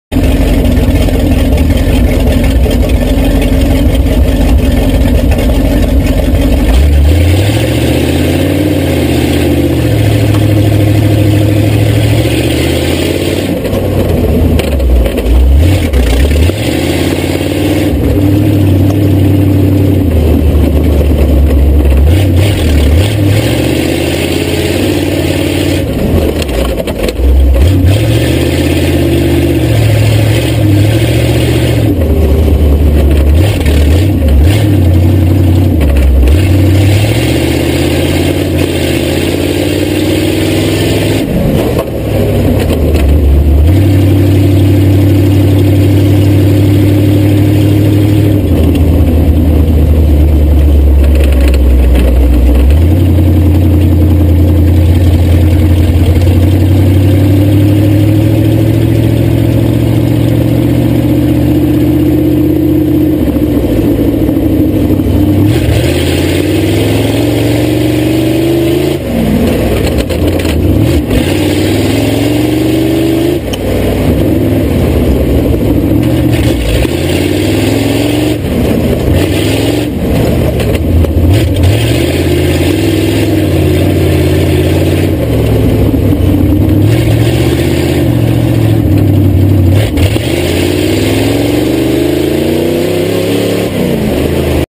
Underneath the Dundirtcheap
A couple laps at the home track with the new Gopro